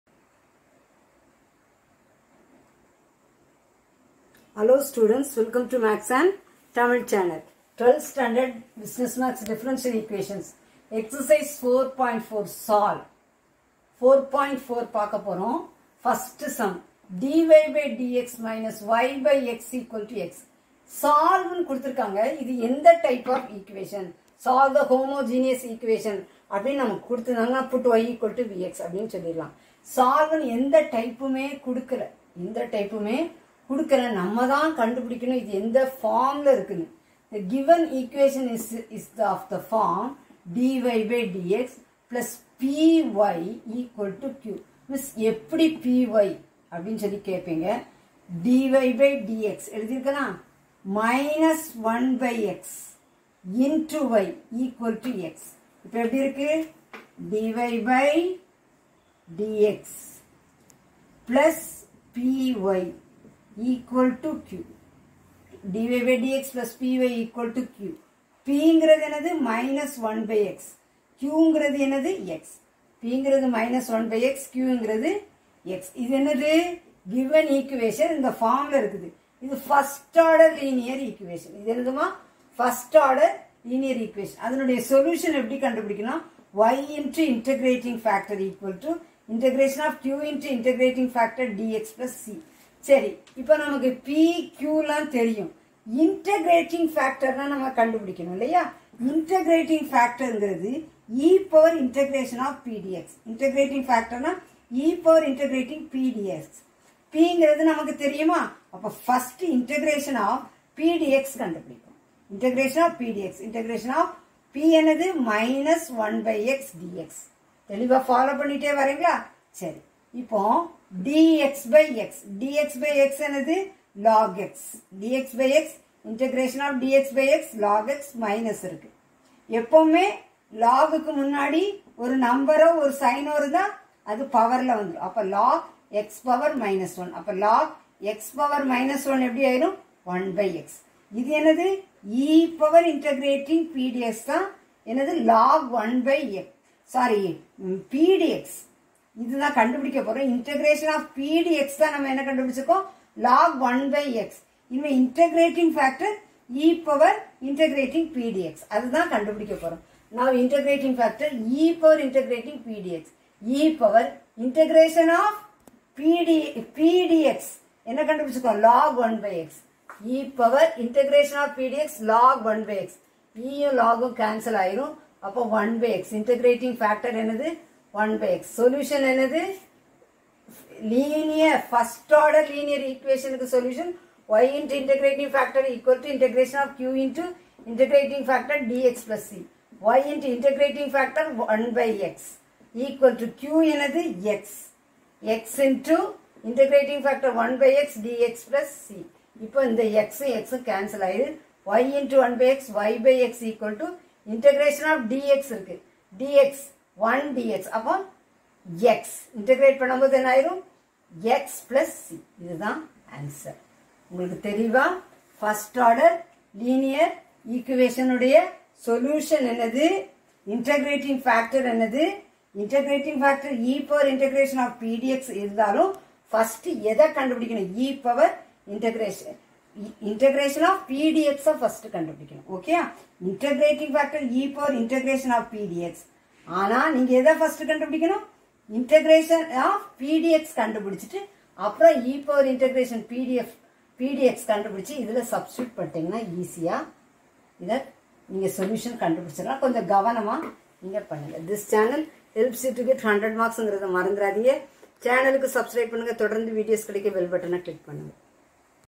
T.N. 12th Business Maths Exercise:4.4 Sum - 1 | Differential Equations | Chapter - 4.
.Dear Students,
New Syllabus
Differential Equations  Chapter - 4

tn12thbusinessmathsex:4.4-1#tn12thb.m.ex:4.4-1#ex:4.4-1#mathsandtamil